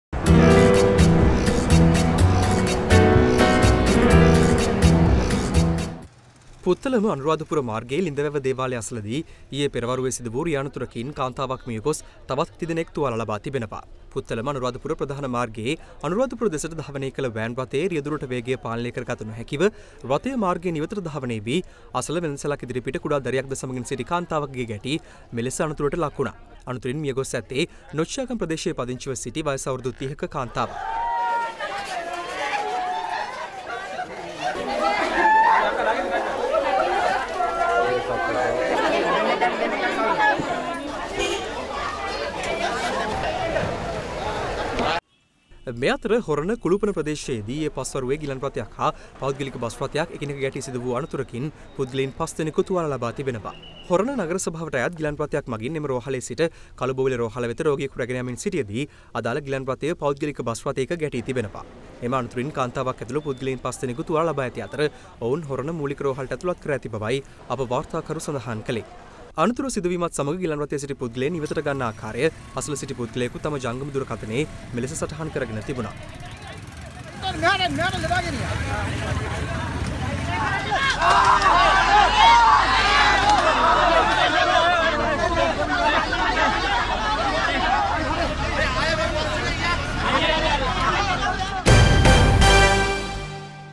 30-year-old mother dies in crash on Puttalam-A’pura road
30-year-old mother dies in crash on Puttalam-A’pura road

පුත්තලම-අනුරාධපුර මාර්ගයේ වෑන් රථ අනතුරෙන් 30 හැවිරිදි මවක් මරුට...